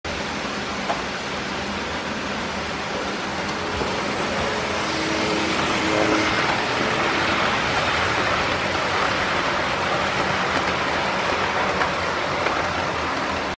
Model Railway: Midland Mainline HST 43070 leads 43069 pass Nottingham with a ECS to Leeds
Model Railway: Midland Mainline High Speed Trains class 43070 leading classmate 43069 seen passing though Nottingham Railway Station while working an ECS from Boston Sidings to Leeds via Nottingham & Sheffield on Saturday 08 of June 2024